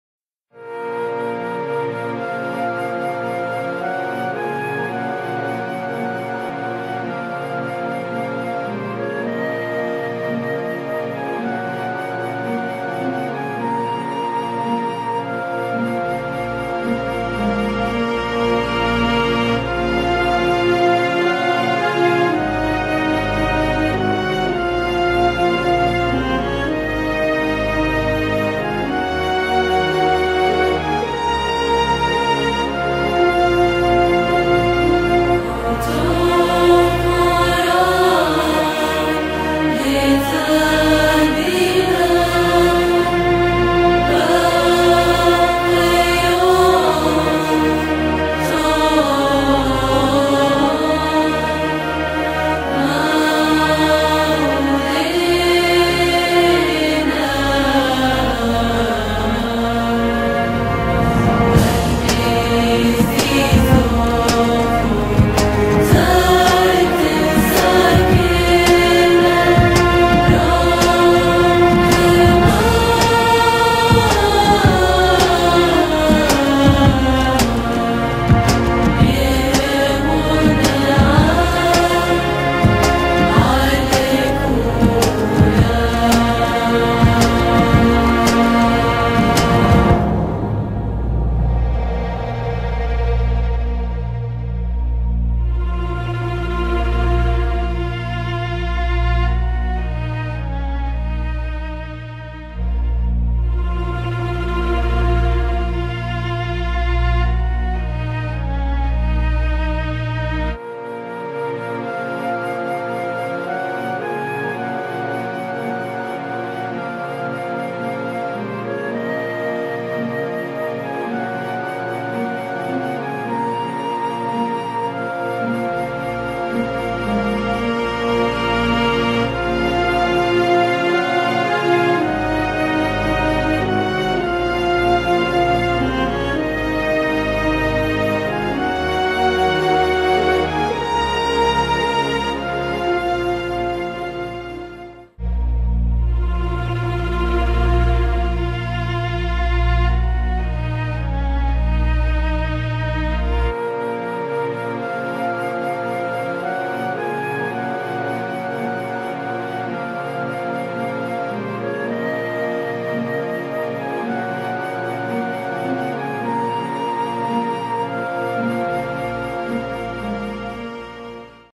ikon.mov
Painting my first real icon painting in Greece